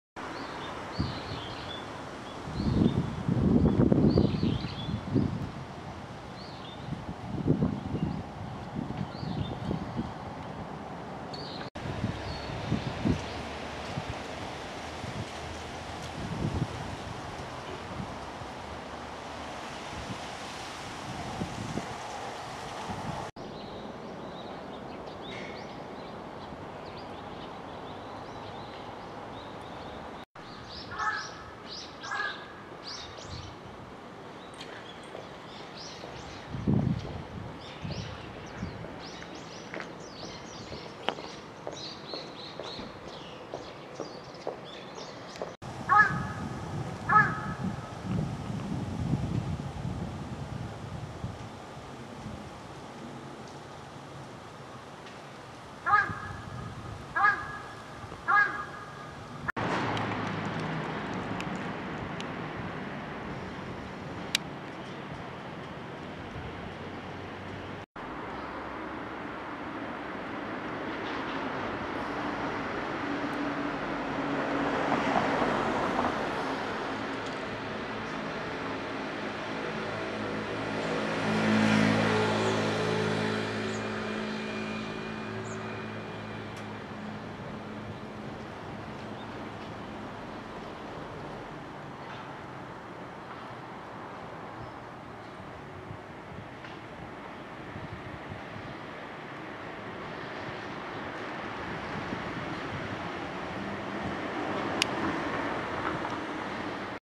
赤羽台団地 建替え工事中
2012年11月4日　建替え工事中の赤羽台団地を撮影してきました
9～12号棟、15号棟、45～48号棟と閉鎖が決まった赤羽台団地商店街の告知